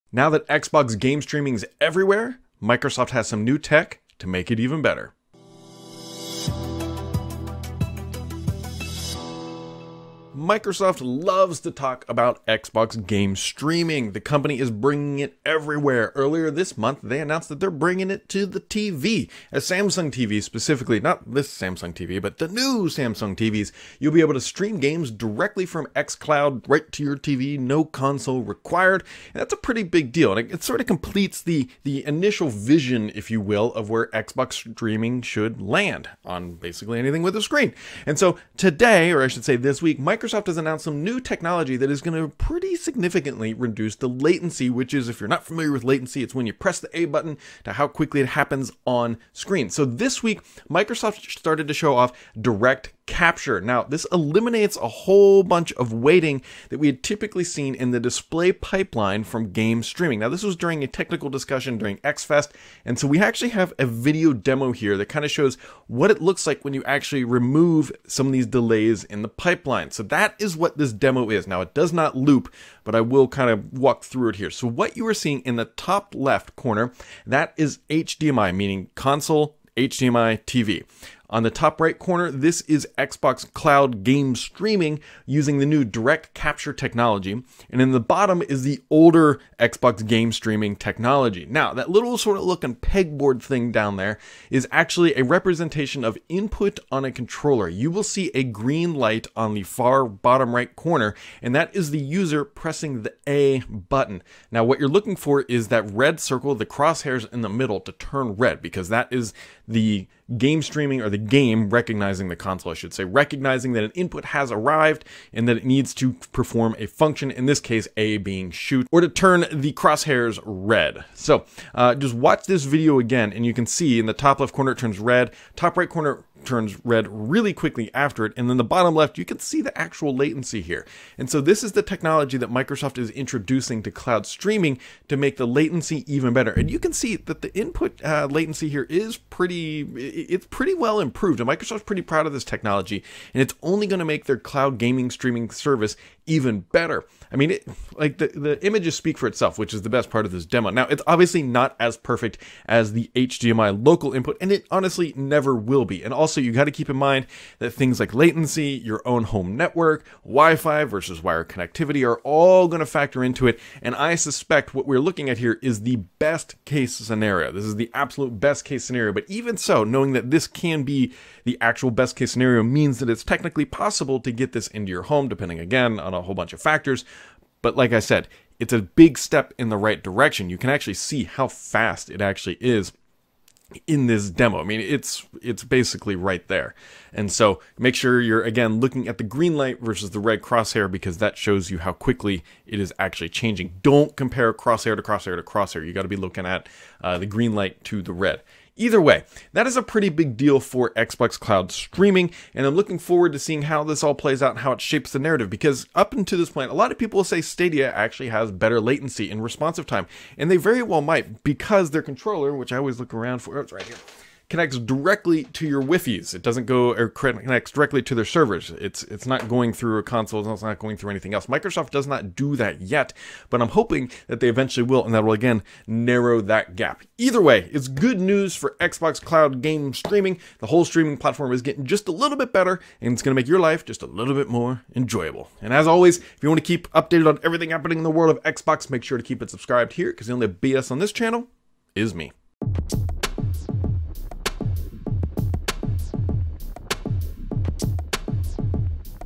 Xbox Game Streaming's Big Upgrade
Microsoft has shown off new technology for Xbox Game Streaming that reduces the latency significantly and should make for a more enjoyable streaming experience.